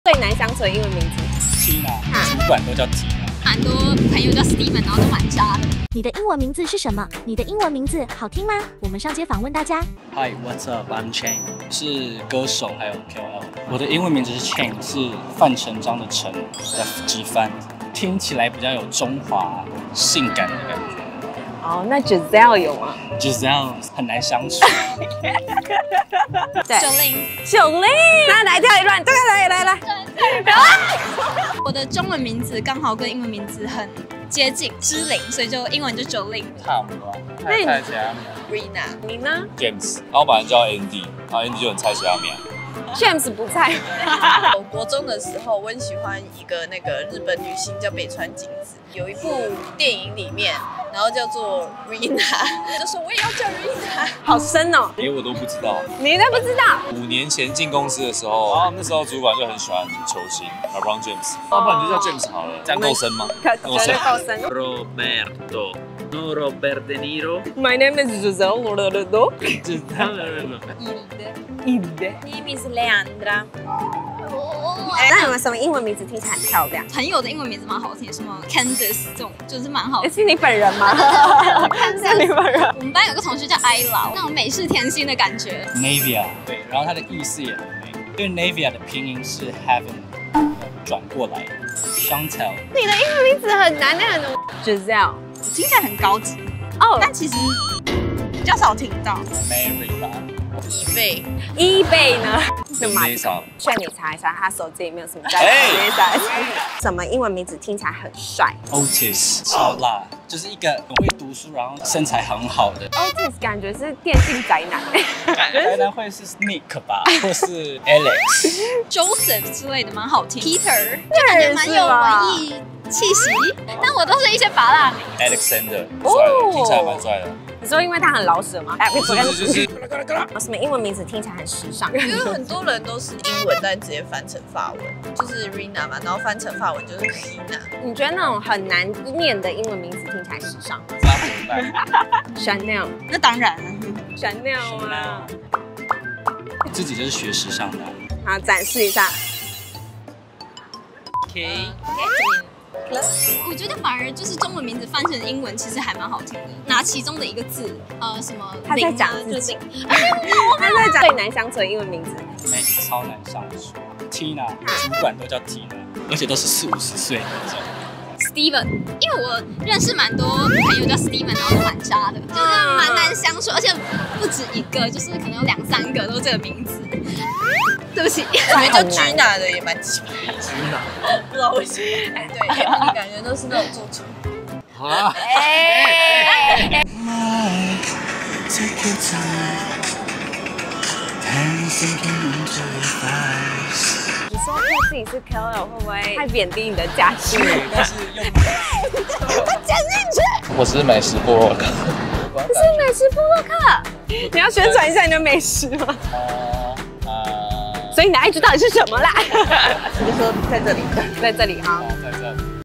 英文名字原來這麼重要？！OTIS聽起來超帥、Tina聽起來很機車？！｜PopDaily 波波黛莉
Cindy、Tina、Alex...
英文名百百種..
有些是一叫會有十個人回頭的菜市場名
有些是一聽到這個名字
就覺得 哇！一定很難相處
原來英文名字也會有刻板印象😎

本集居然還有歌手跟美食部落客？！
你的英文名字是怎麼來的呢？
\歡迎留言跟我們分享 ‎･ ͜ʖ ･/

0:14 英文名字由來
0:27 很配合的9令💃
1:05 （只吃過義大利麵..在線求翻譯）
1:15 很漂亮的英文名？
1:47 聽起來很帥的英文名？
2:10 很時尚的英文名？
2:34 9令偷偷稱讚自己
2:41 最難相處的英文名？
3:04 幕後花絮!歌手獻唱🎤

感謝
訂閱『 PopDaily 波波黛莉的異想世界 』&開啟通知🔔
⬇️掌握最新影片⬇️
波波街地趣｜波波匿名聊｜
PopDaily 波波黛莉的異想世界